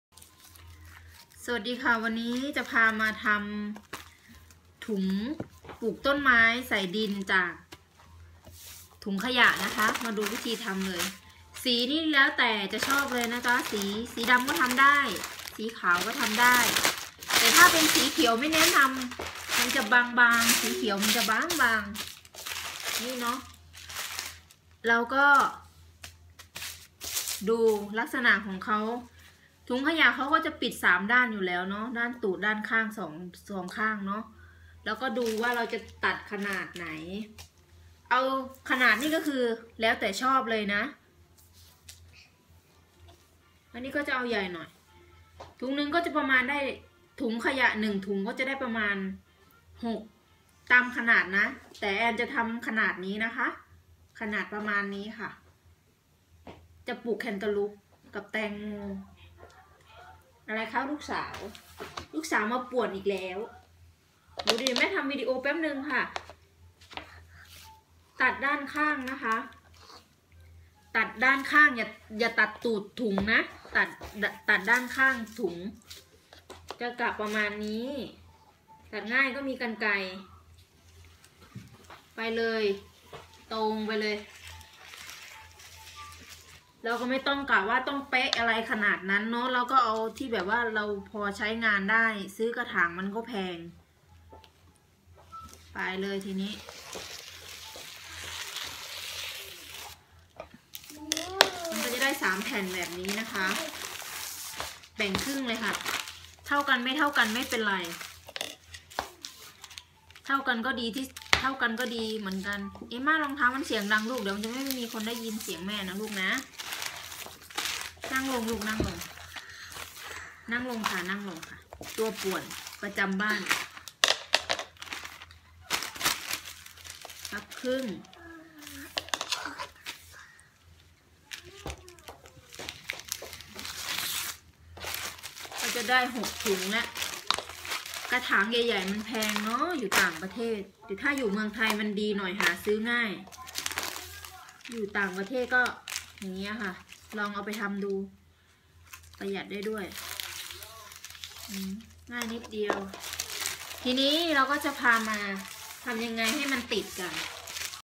วิธีทำถุงเพาะชำ จากถุงขยะคะ
อุปกรณ์ก็มี ถุงขยะ เตารีด ผ้ารองรีด แผ่นฟอยด์กันความร้อนค่ะ